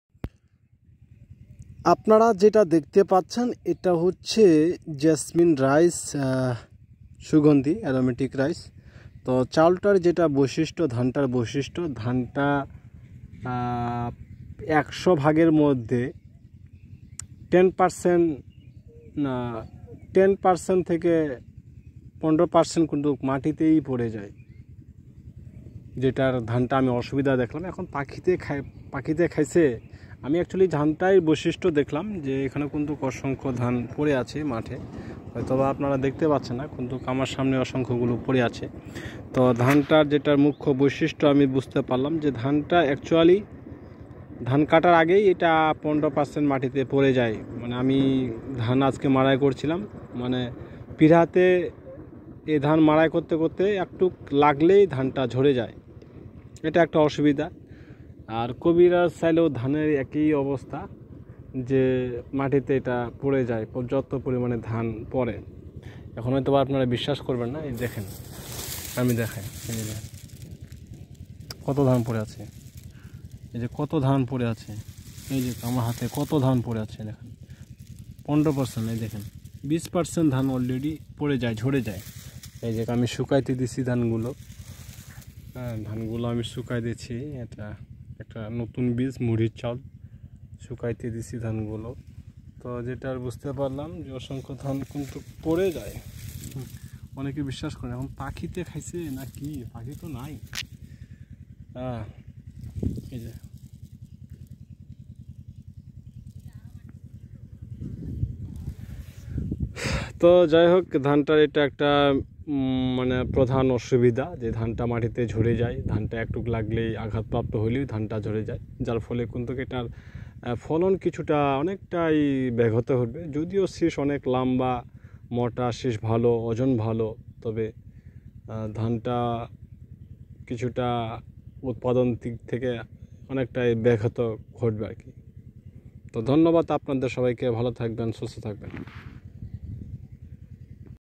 জেসমিন রাইস চাষাবাদ করে যেটা বুঝতে পারলাম ধানটা পাঁকার সাথে সাথে কিছুটা ঝরে যায় জমিতে।
জেসমিন রাইস সুগন্ধি একটা ধান । জেসমিন রাইস লম্বা ও চিকন মোটা। ধান